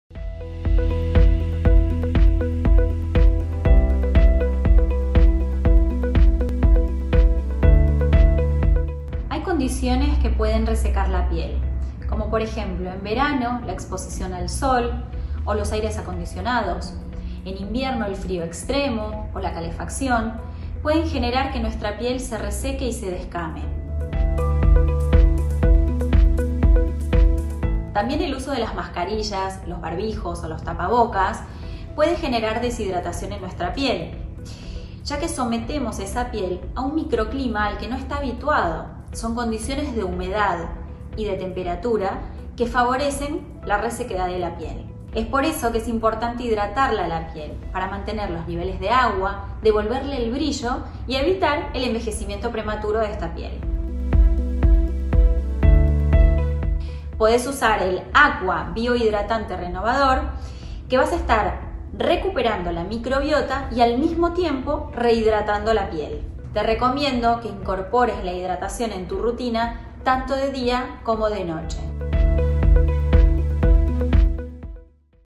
Condiciones que pueden resecar la piel
Ten conocimiento de cuáles son y cómo cuidar tu piel en estas situaciones.

Capacítate y aprende todo sobre Natura, sus productos y el negocio